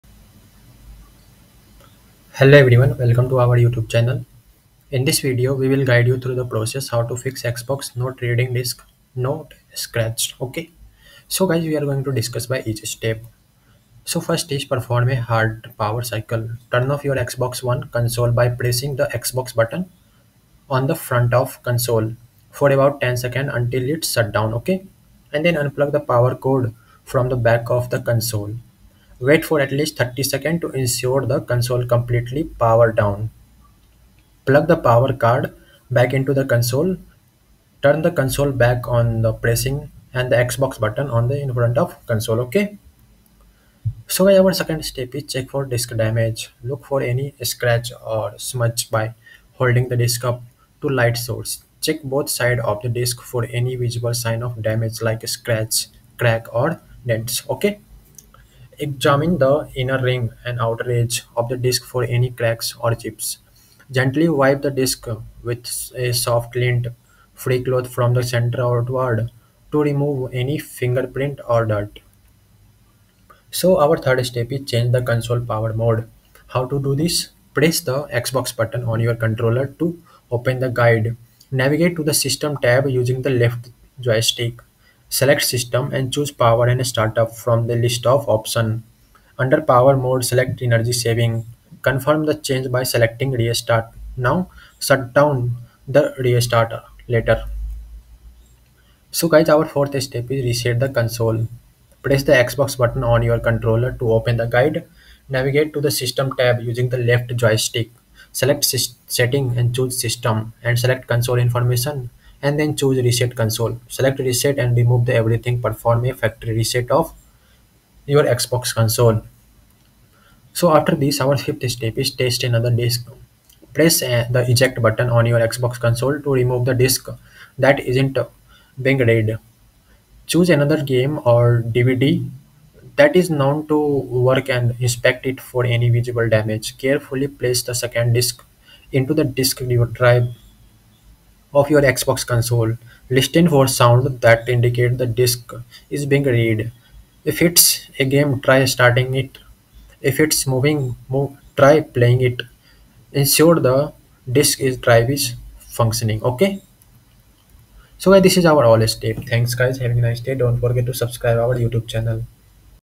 How To Fix Xbox Not Reading Disc No Scratches
Hello everyone welcome to our youtube channel In this video, you will learn how to troubleshoot and fix the issue of your Xbox not reading discs, even when there are no visible scratches. Follow the step-by-step guide to resolve this common problem and get back to enjoying your favorite games in no time.

00:15 Fix 1: Perform A Hard Power Cycle
00:44 Fix 2: Check For Disc Damage
01:14 Fix 3: Change The Console's Power Mode
01:40 Fix 4: Reset The Console
02:01 Fix 5: Test Another Disc